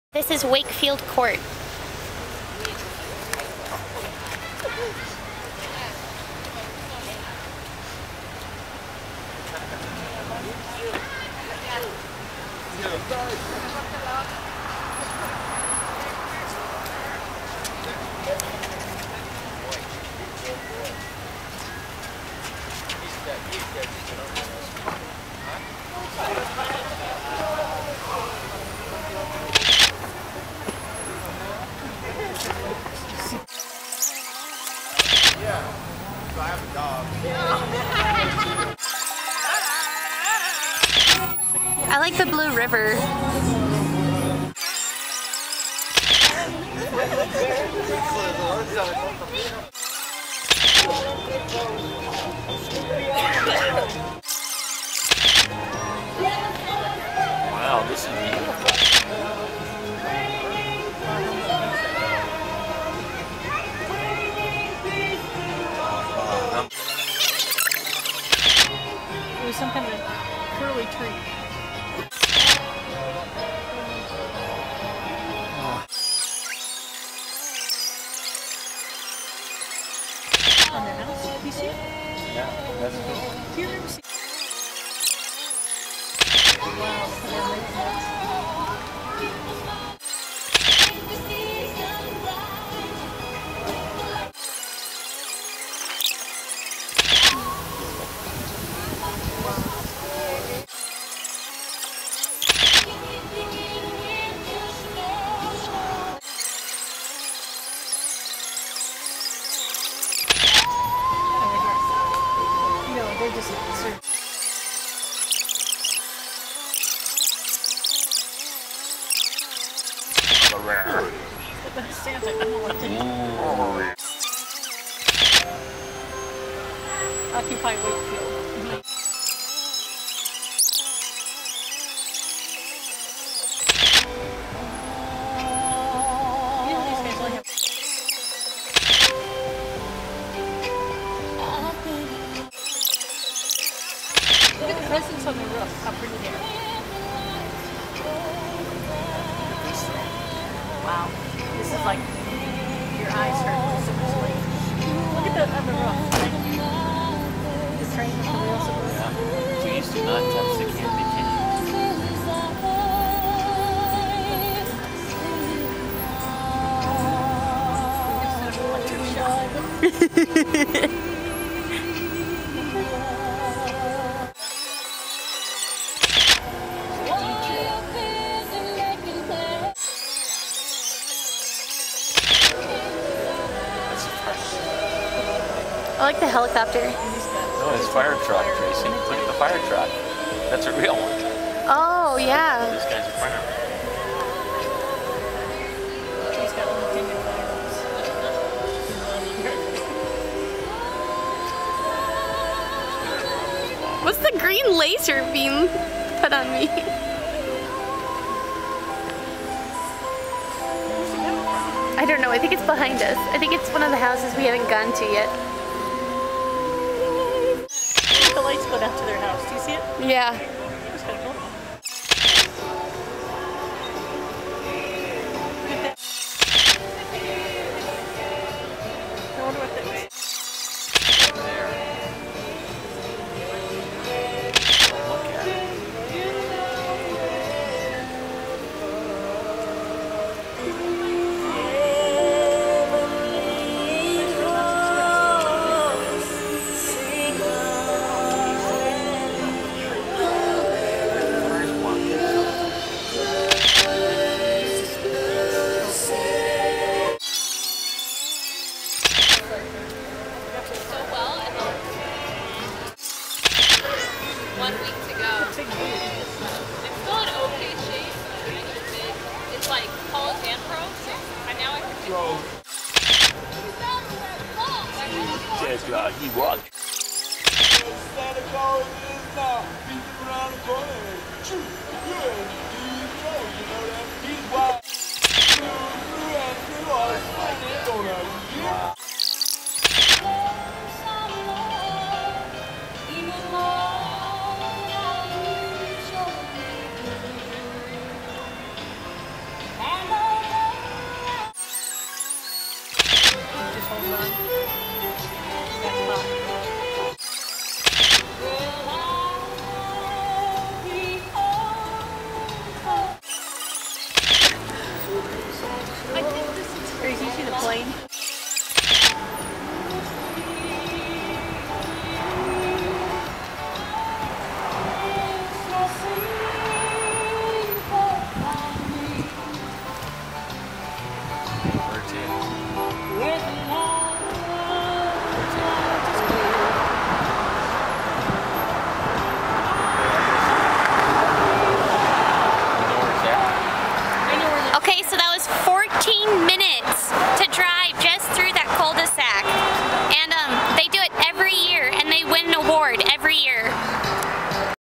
Wakefield Court Christmas Lights
This is a spectacular Christmas Light display put on every year by Wakefield Court in Santa Clarita, California.